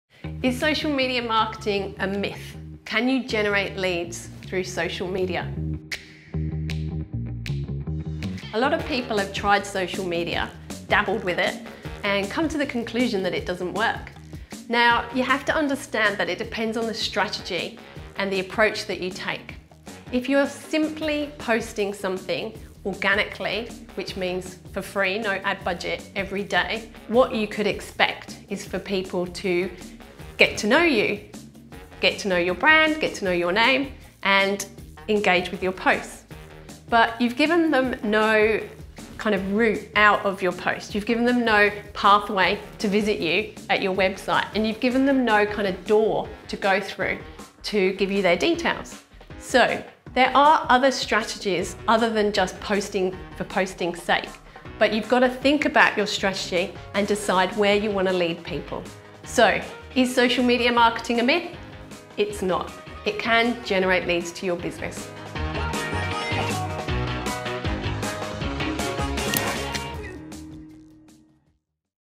Is social media marketing a myth?
Is social media marketing a myth? Can you generate leads through social media? The answer is simple, you just have to think about how and where you want to lead people. Listen to my one minute marketing tip.